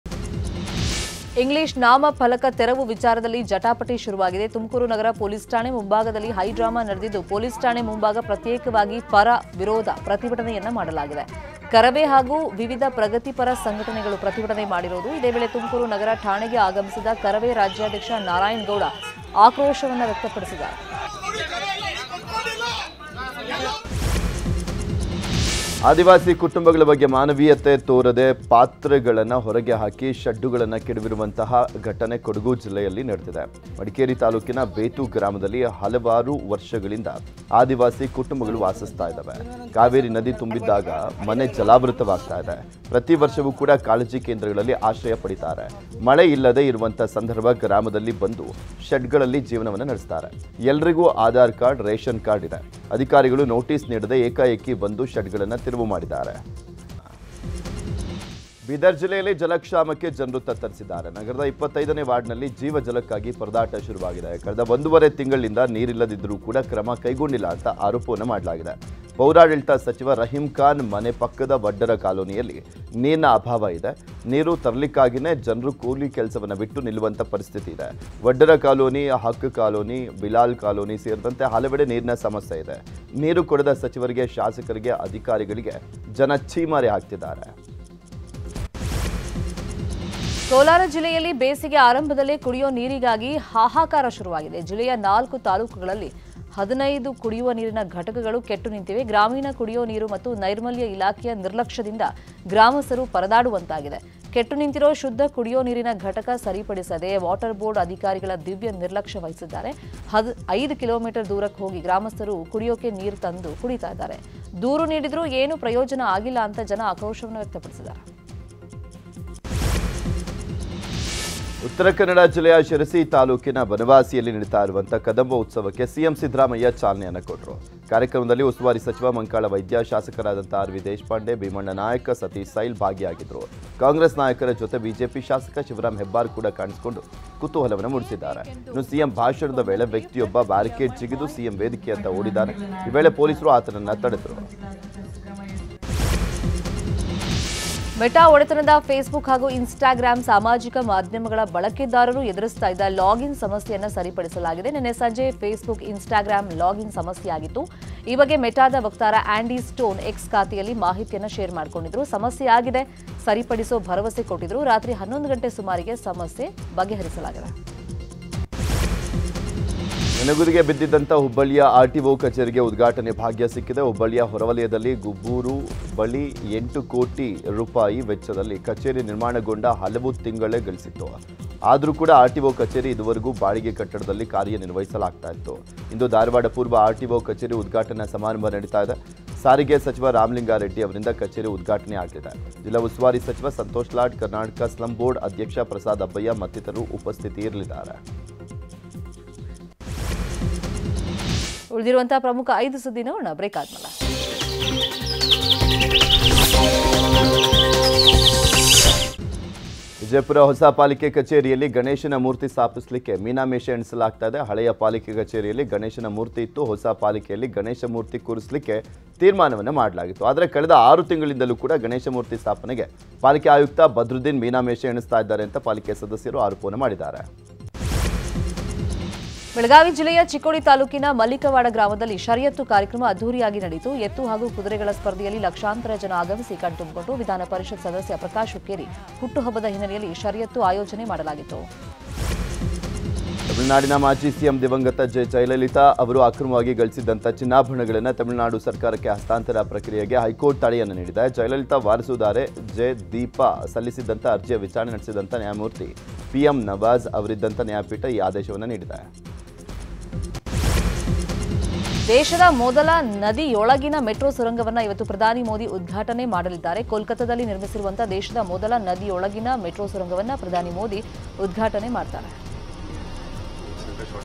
Atrocities On Tribals | ಆದಿವಾಸಿಗಳ ಮೇಲೆ ದೌರ್ಜನ್ಯ! ನೋಟಿಸ್ ನೀಡದೆ ಏಕಾಏಕಿ ಬಂದ ಶೆಡ್‌ಗಳ ತೆರವು | Madikeri
Atrocities On Tribals | ಆದಿವಾಸಿ ಕುಟುಂಬಗಳ ಬಗ್ಗೆ ಮಾನವೀಯತೆ ತೋರದೆ ಪಾತ್ರೆಗಳನ್ನು ಹೊರಗೆ ಹಾಕಿ ಶೆಡ್ಡುಗಳನ್ನು ಕೆಡವಿರುವ ಘಟನೆ ಕೊಡಗು ಜಿಲ್ಲೆಯಲ್ಲಿ ನಡೆದಿದೆ.

News18 Kannada Live | ನ್ಯೂಸ್ 18 ಕನ್ನಡ ಲೈವ್
Karnataka | ಕರ್ನಾಟಕ
Kannada | ಕನ್ನಡ